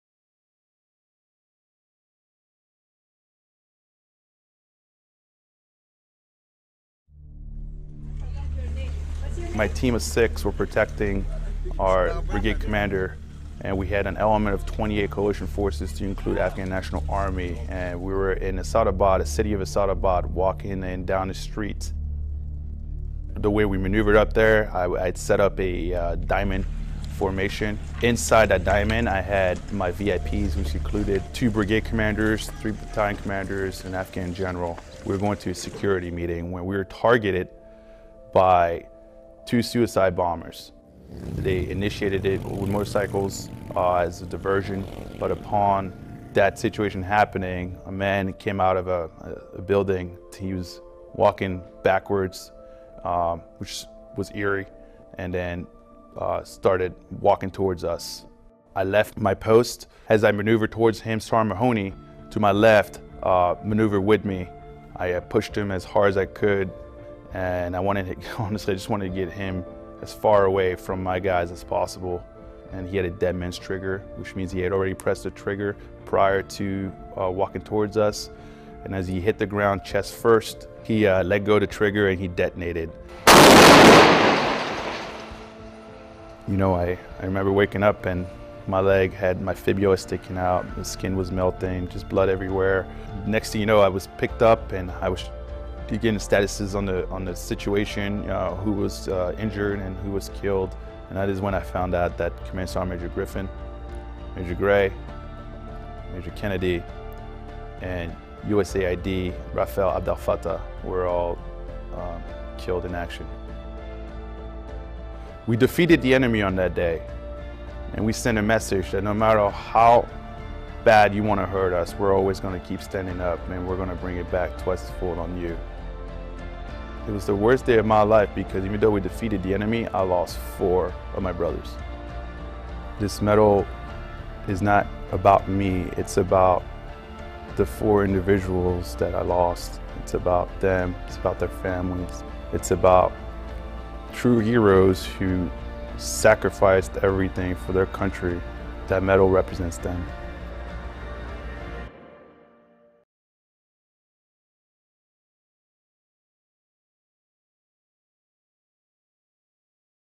Florent Groberg's Medal of Honor Story
October 2015 - Retired Capt. Florent "Flo" Groberg talks about the incident in Kunar Province, Afghanistan, Aug. 8, 2012 with associated scenes. As a result of his actions, Groberg sustained the loss of 45 to 50 percent of his left calf muscle with significant nerve damage, a blown ear drum, and a mild traumatic brain injury.

Video courtesy of U.S. Army

USA Patriotism!